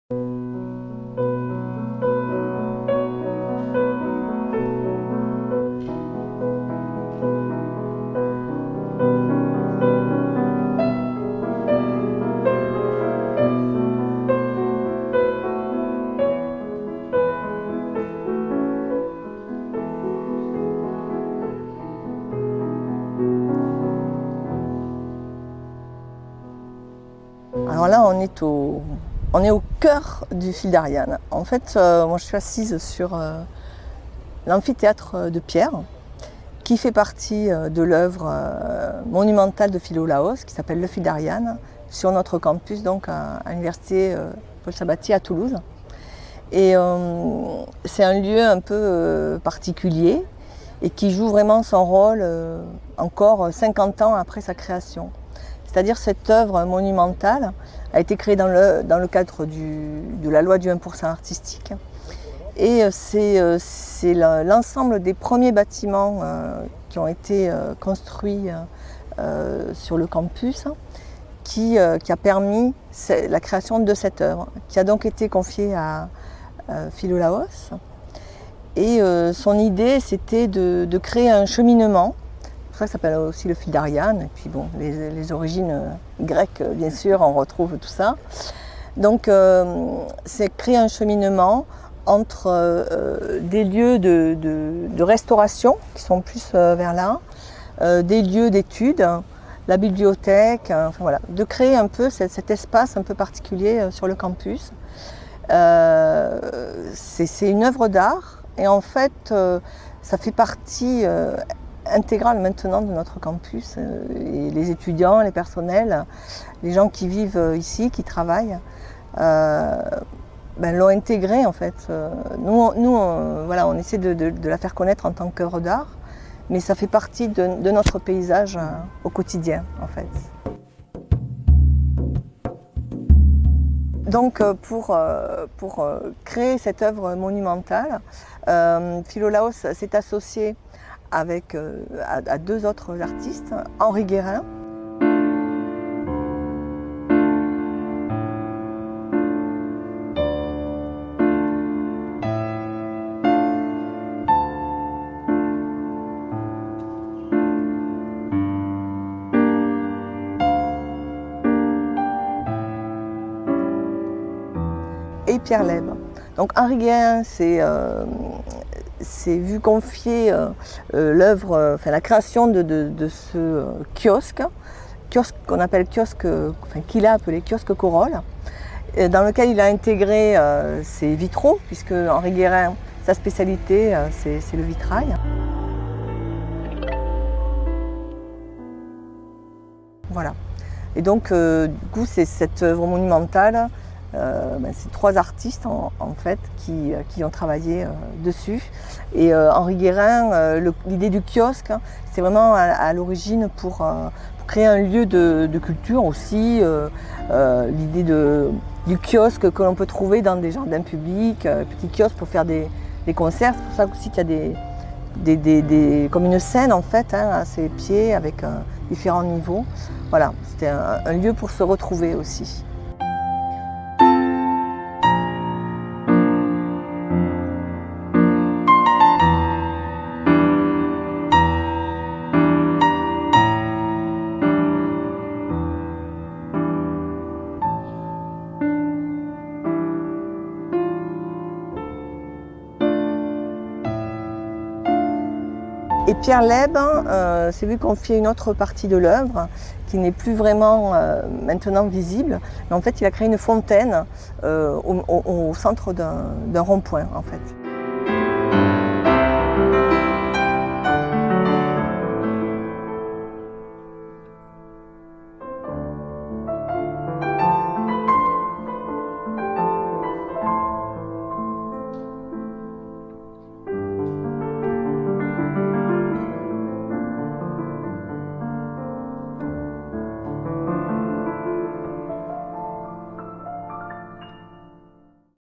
Philolaos – O Μίτος της Αριάδνης
"O Μίτος της Αριάδνης" ένα υπαίθριο αμφιθέατρο και ένα μονοπάτι με γλυπτά στο Πανεπιστήμιο της Τουλούζης.